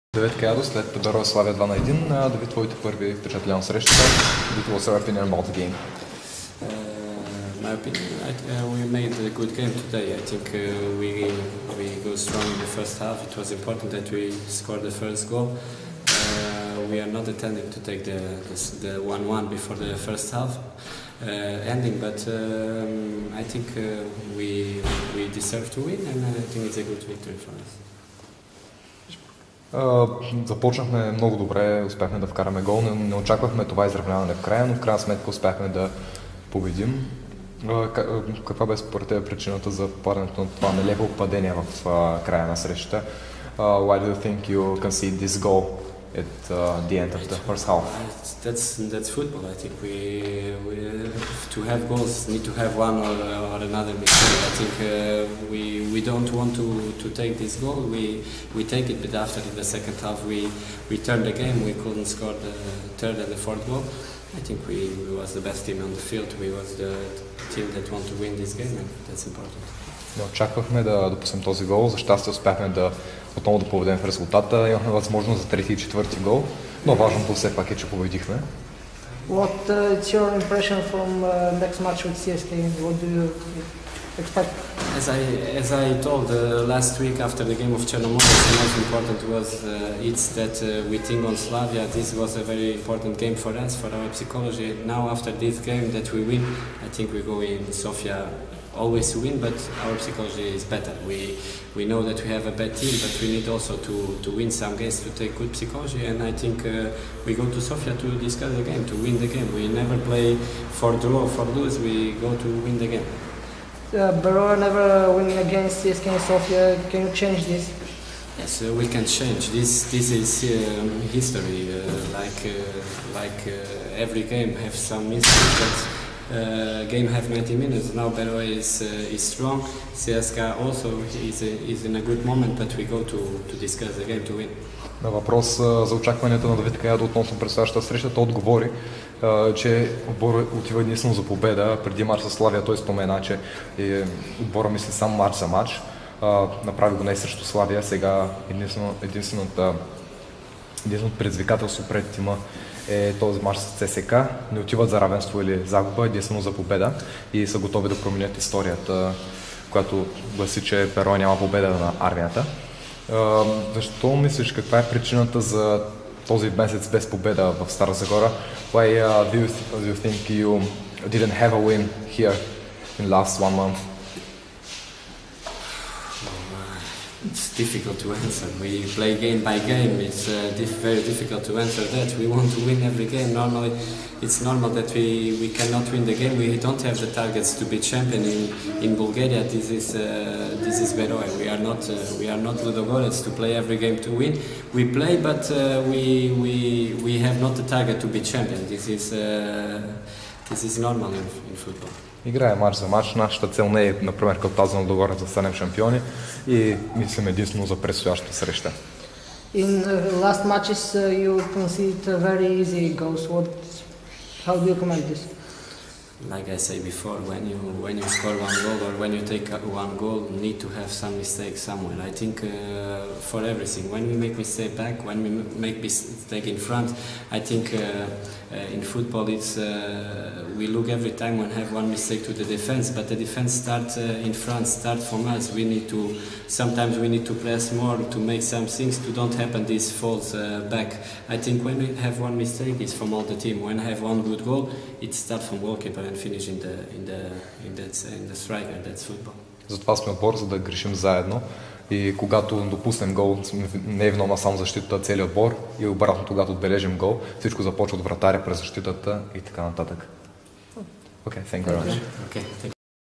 Давид Каядо след Берое - Славия 2:1
Давид Каядо отбеляза и двата гола в срещата със "Славия" и излезе начело в голмайсторската листа на "Берое". След мача той бе любезен да говори за beroe.tv относно впечатленията си от двубоя. Вижте какво сподели крилото.